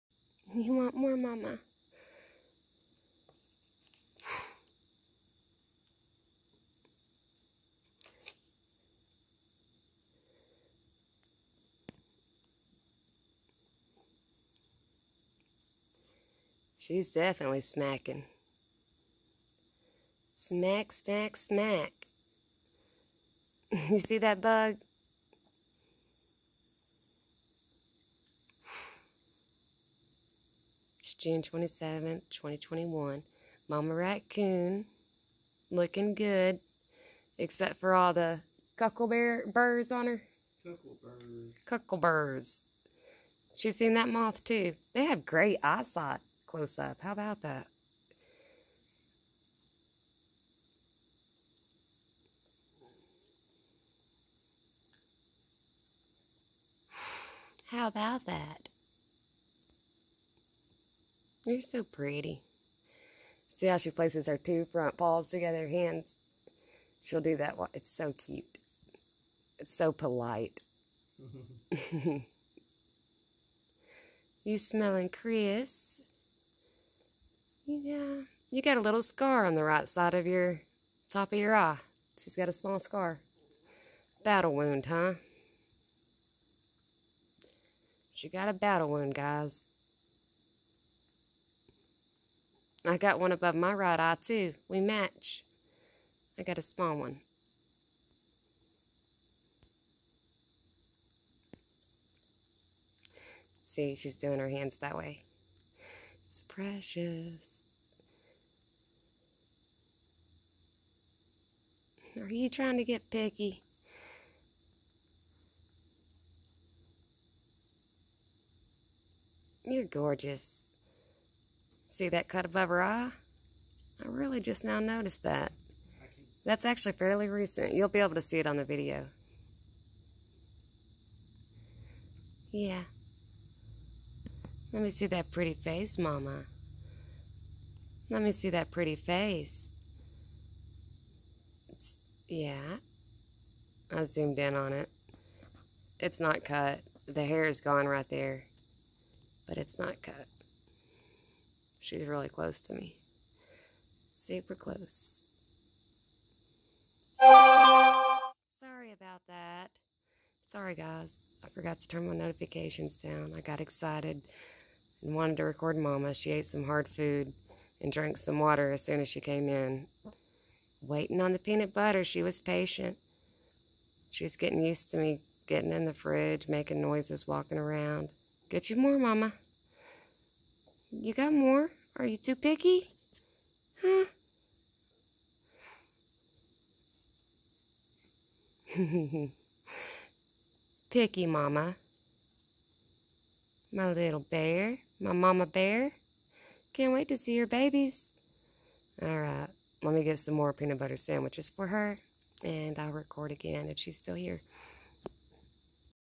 Raccoon 💖 Mama June 27th 2021 11:00 p.m.
Welcome to our country life! 😊❤🦝💗 I hope you all enjoy them as much as we do!! 💕

I really love hearing about everyone else's experiences with these sweet babies! All the pets! From all around the world! 🌎💞 It truly is a great experience! 💗💕🦝

We have already found ways to make sure they are good in the winter time! No worries 💕

Every night for the last few months, a mother raccoon 🦝💕 and five of her babies come by to eat for about 15 to 30 minutes. Like going to a diner. They are not dependent on us, but the mother was really struggling and her babies as well to gain weight. It is very nice they get to actually dine in whenever they feel like it. They live 30 ft behind our garage in the top story of our barn. They have not harmed anything at all and they are so adorable, awesome, interesting, calming, loving, gentle, polite and so on! 💞 We have woods, 🌳🌲🌳 a creek 🏞 on the back of our property and a pond about 75 feet from the barn, so they get a lot of their natural diet, we seen it in their scat! 😄 They have gained so much weight, we have learned all their personalities and different looks! Which I think is pretty awesome & cool 😎! Our entire family enjoys them and our friends. They love to stop by our diner! On the menu hard food, hot dogs, grapes, peanut butter, granola bars, bread and we tried bananas once but they did not take to it. We really love to get to know these guys, we get to spend about 15 minutes with them, being able to pet them, love on them and show them love! 💖😍 They are absolutely beautiful creatures! 😍 ❤ I have been around them my entire life off and on, at different ages of their lives. I hope you all enjoy them as much as we all do! 💞 Mama, Zorro, Tico are the untameables! Runt Runt aka Runty, Buddy & Fluff Fluff aka Fluffy are the tame ones! ❤